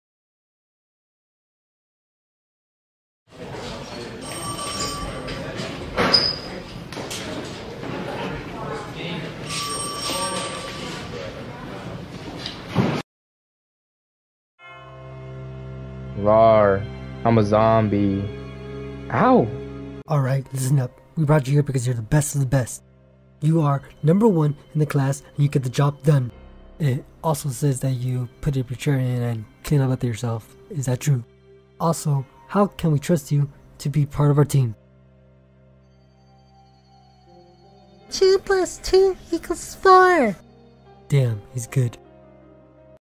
The Trust
A small animated cartoon.
We have Officer Gustavo and Detective Noris giving an interview to the best of the best!
Voices:
Detective Noris - Ferny
Best of the Best - Ferny
Zombie - King